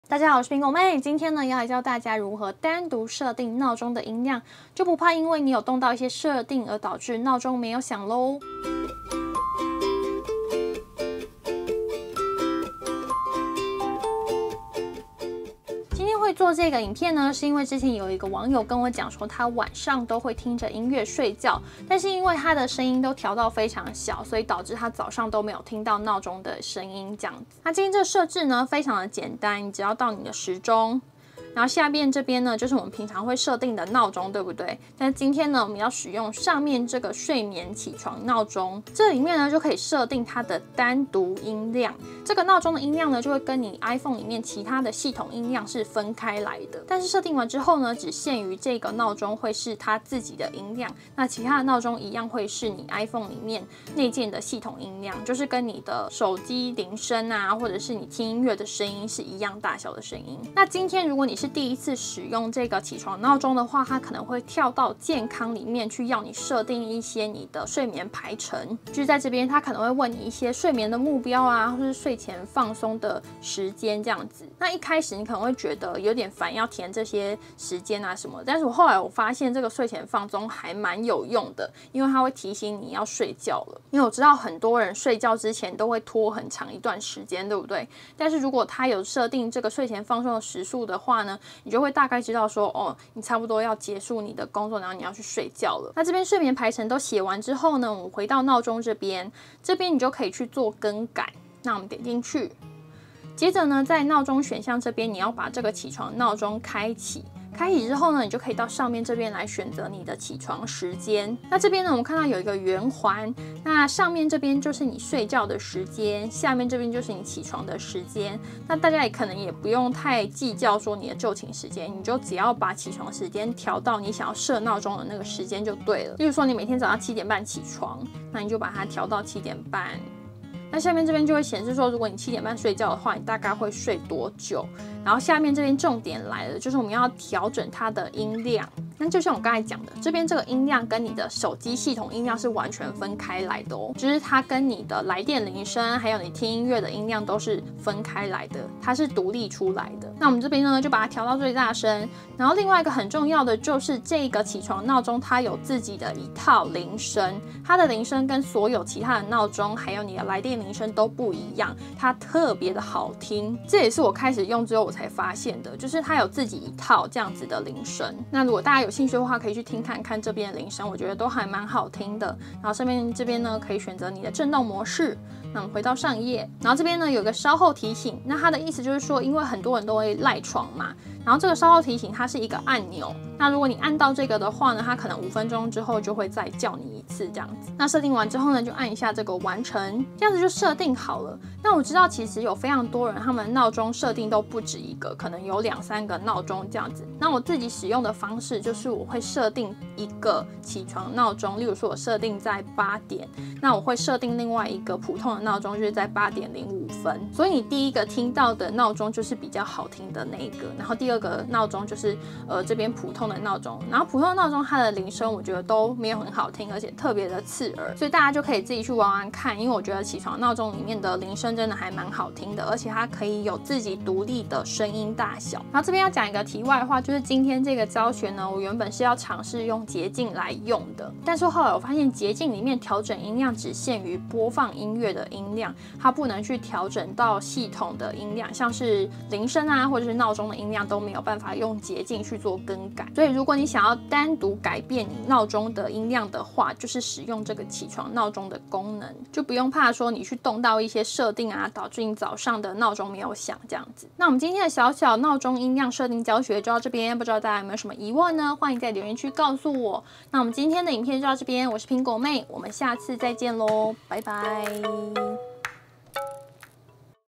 睡過頭了！iPhone鬧鐘小技巧⏰ 獨立音量 iOS實用教學
常常害怕動到音量導致早上聽不到鬧鐘嗎？今天要來教大家一些iPhone鬧鐘小技巧，可以獨立設定鬧鐘音量，也有自己一套好聽的鈴聲！